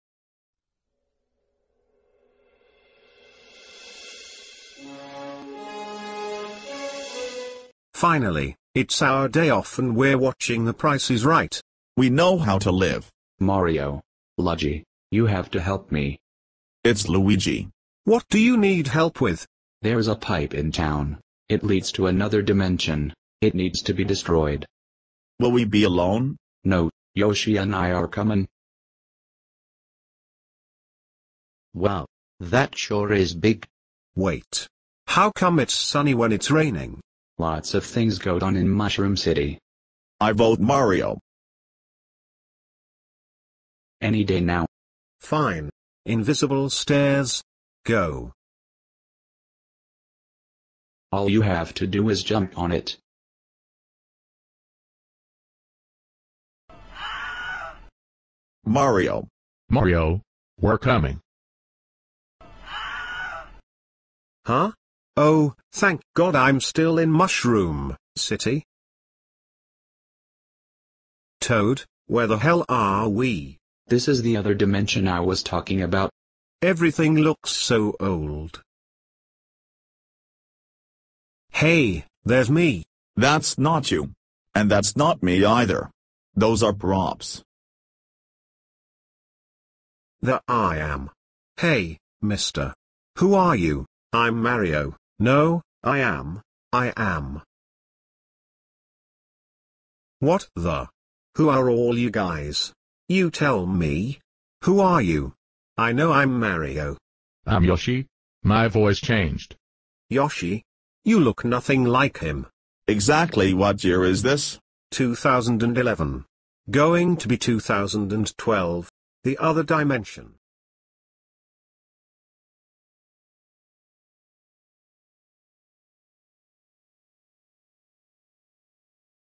Sneak Peek of Super Mario: The Next Generation 3
Mario, Luigi, Toad, and Yoshi get trapped in another dimension